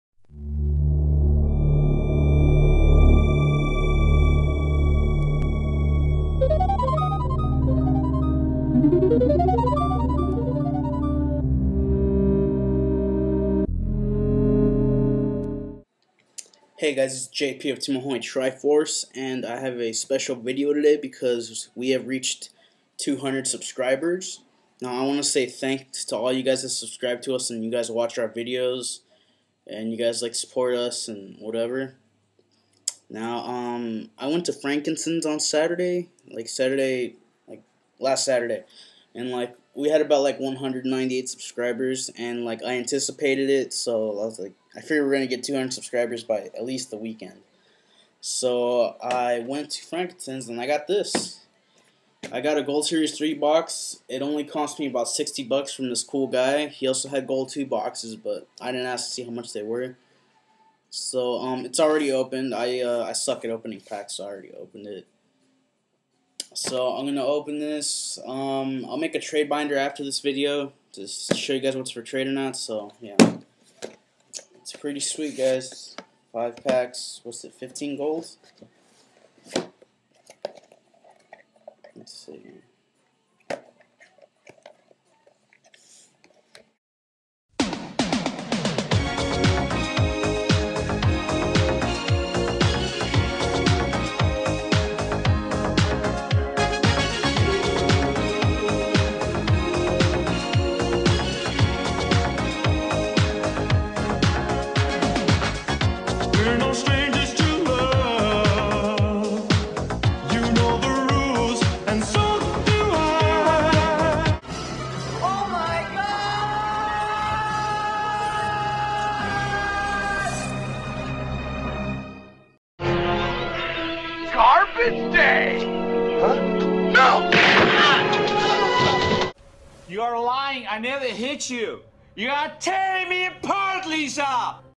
TeamUnholyTriforce 200 sub specia!!!!l Gold Series 3 Box opening
hey guys this is Jp with a Box opening for you guys, i open a gold series 3 box with EPIC PULLS

ps DON'T SPOIL THE SURPRISE